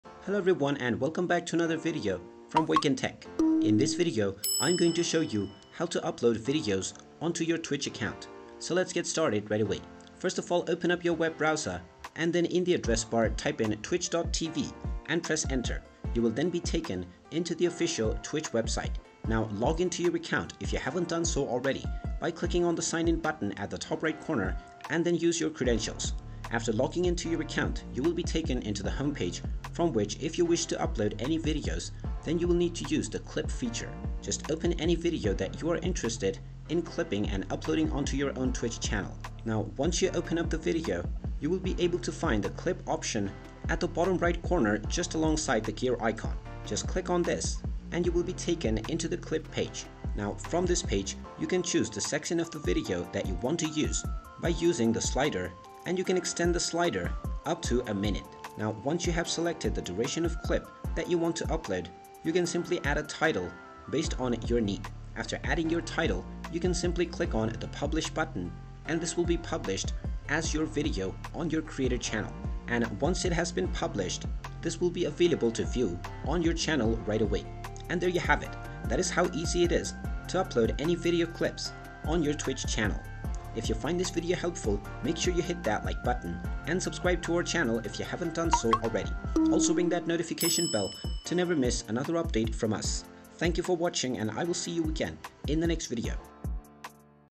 How to Upload Videos on Twitch 2024?
Ready to showcase your gaming highlights, tutorials, or vlogs to your Twitch audience? In this step-by-step guide, we'll show you exactly how to upload videos to your Twitch channel, allowing you to share your content and engage with your viewers in a whole new way.